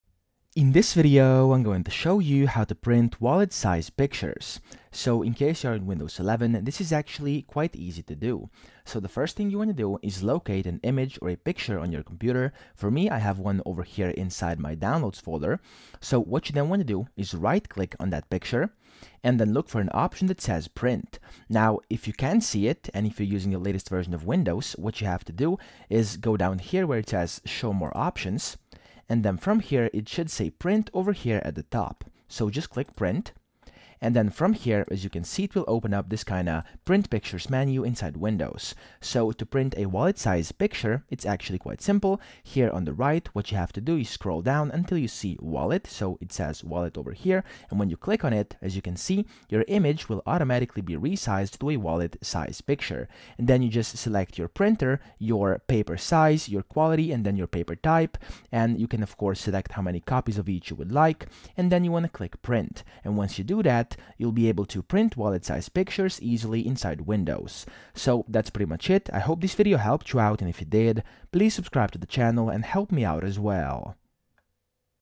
How To Print WALLET SIZE Pictures (QUICK & EASY) 2026
In this video, I'll show you how to print wallet size pictures in windows 11. This is a full guide on how to print wallet size images in windows in 2026.

In case you've been trying to print wallet size pictures on computer, this is the video you need to see!